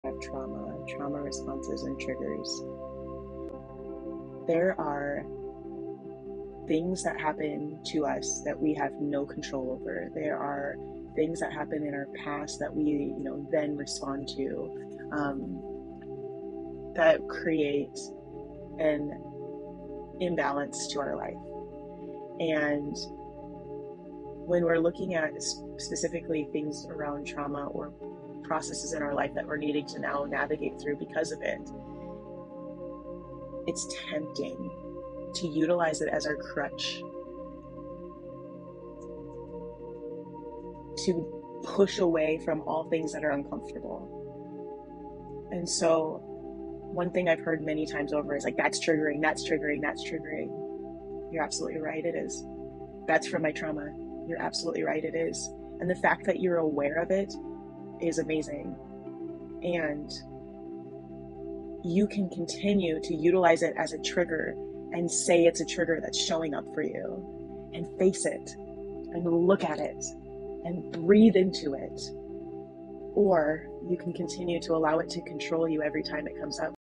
Trauma and Triggers
Trauma and Triggers: In this video, we delve into the impact of trauma and how to navigate triggers with empathy and self-compassion. Join me as we discuss strategies for anxiety relief, managing depression, and finding peace through mindful breathing.